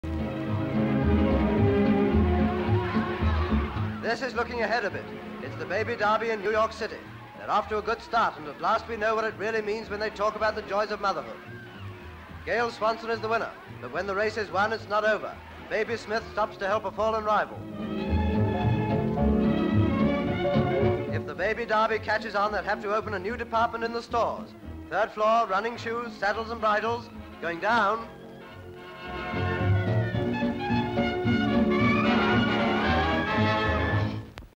A baby derby in America (1940)
Toddlers are numbered and placed at the start line and they crawl for the finish with some tiny tikes loosing their diaper

Full Description:

USA: New York: New York City:
INT

BABIES. Baby crawling marathon:one loses pants.

Competitions

children, United States of America, US, contest
Background: Toddlers are numbered and placed at the start line and they crawl for the finish with some tiny tikes loosing their diaper